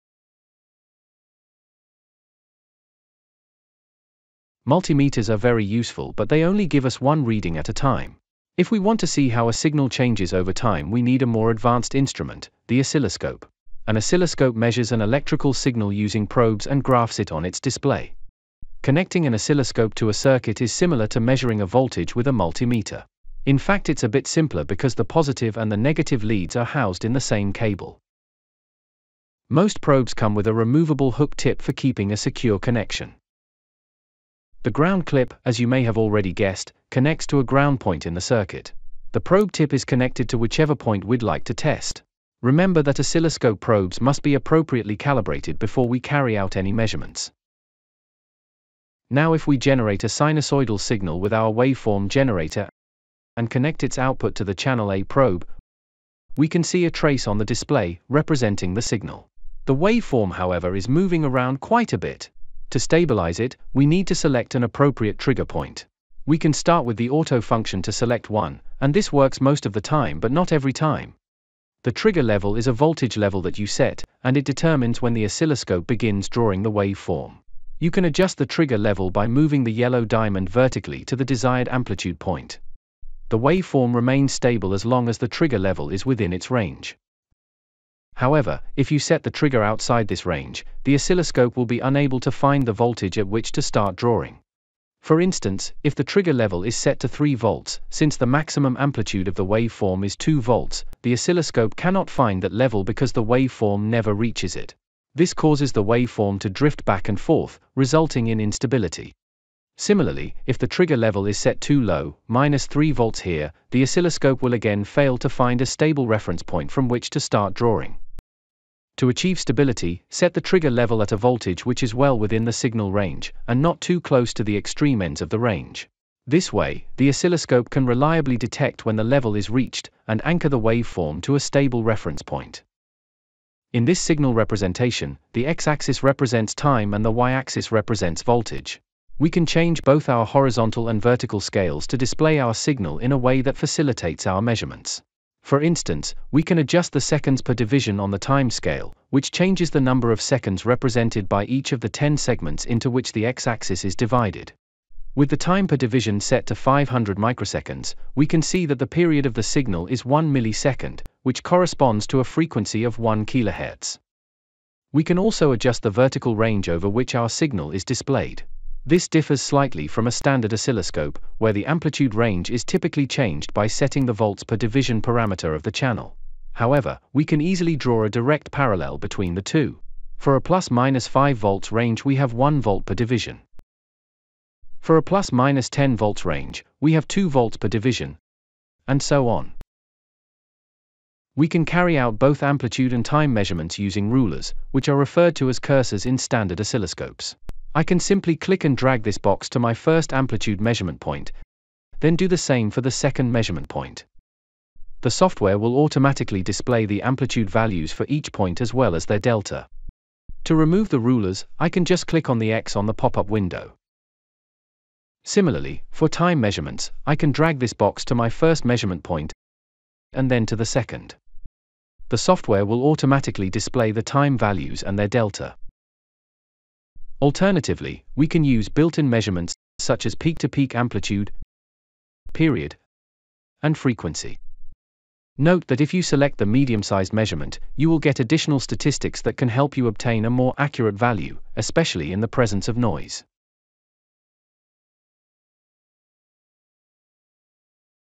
P2 - Oscilloscope's Basics with Picoscope
In this video, we illustrate the basic settings and measurements of an oscilloscope using the Picoscope 2204A. Please note that the probe must be properly calibrated to accurately replicate the actions shown in the video. For a probe calibration tutorial, please refer to this video

This video, along with many more, is featured in our free online course, 'Foundations of Electric Circuits,'